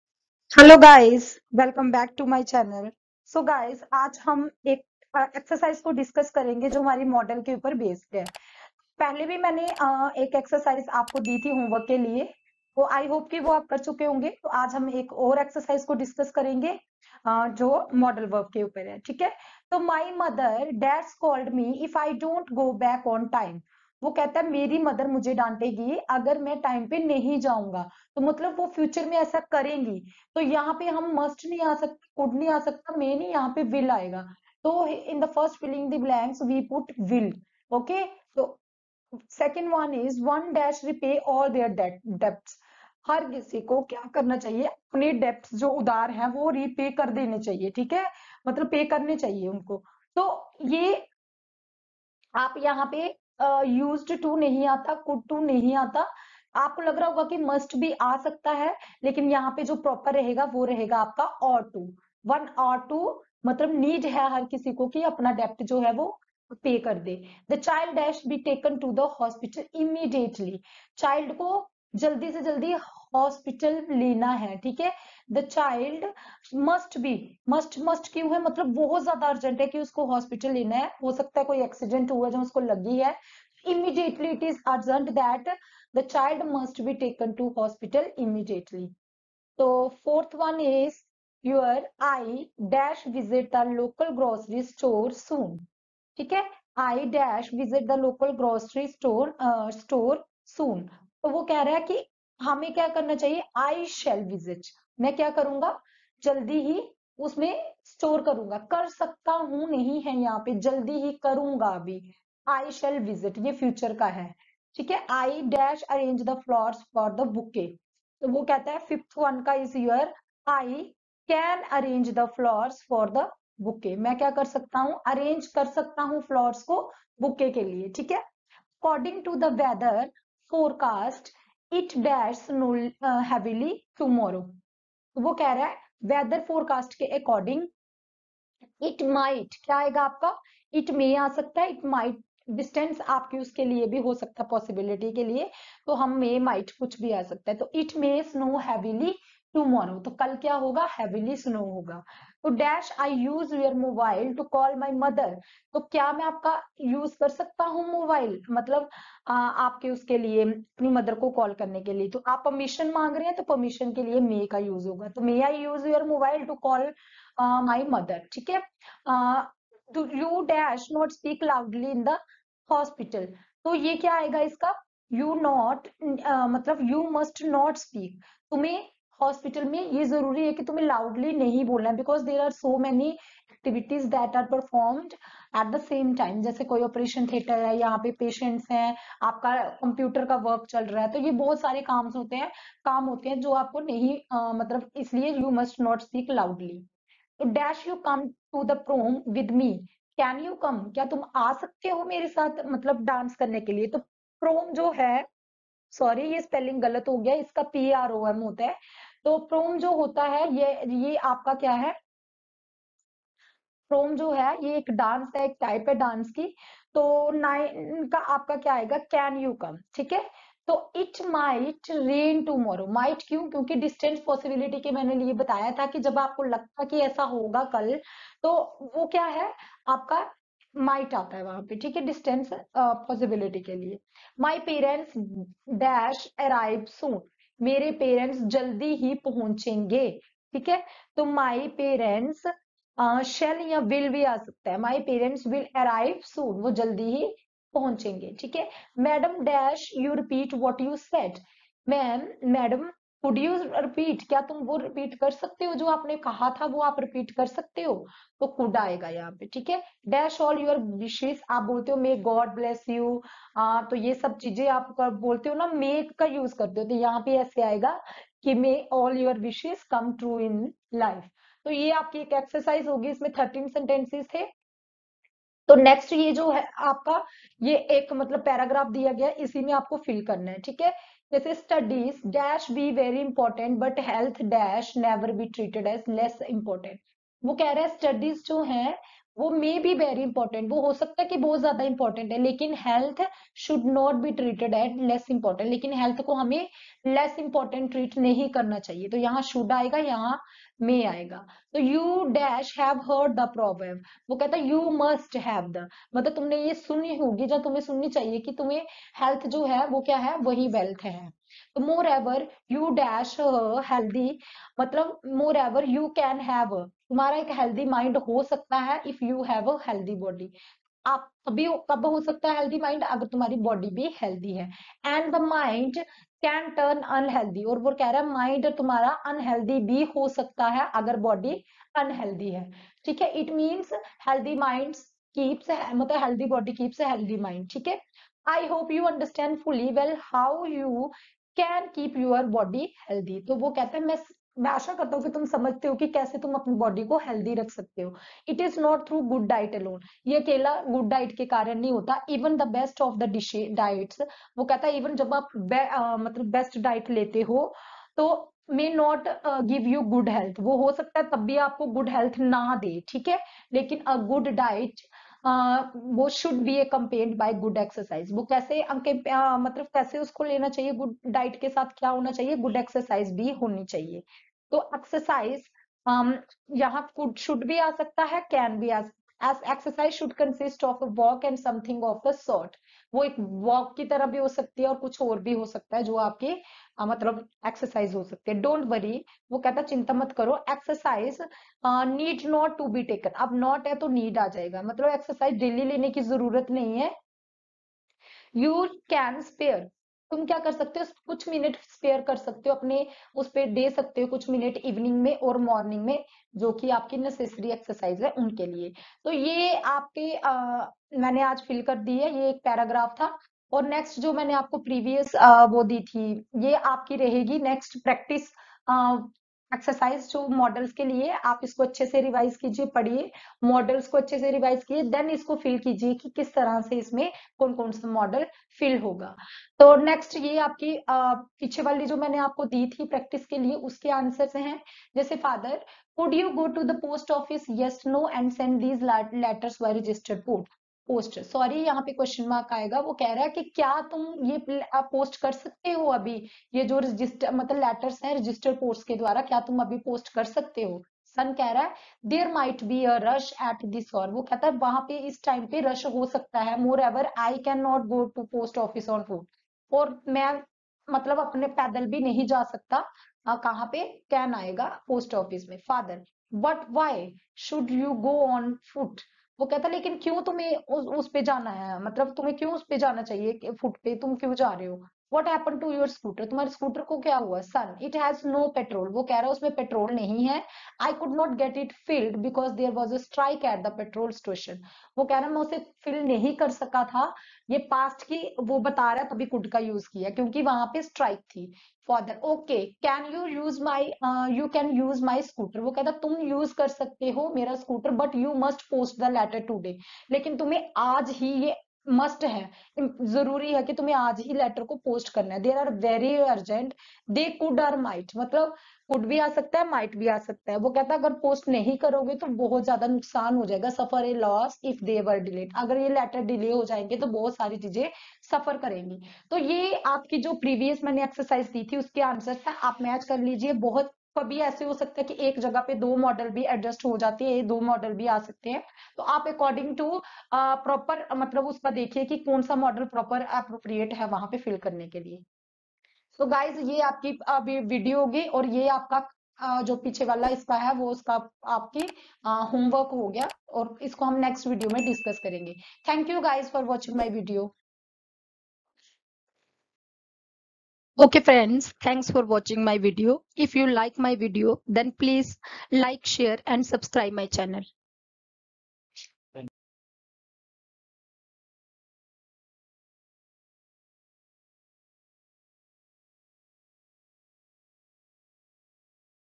Exercise on Modal Verbs:MCQ-2 Modal English Grammar|All Class/Competitive Exam Prepration Student
Modals:Verbs MCQ-1
In this vedio, we  are discussing/presenting some fill in the blanks with appropriate modal verbs  and in the last of this vedio, one unsolved passage for your practice , the answer of this vedio will be discussed in the next vedio or you comment in the comment box i will send  you the passage.

 In the next vedio, we will be discussing the next topic  on your demand.

You will join the grow app by clicking on this link and make more money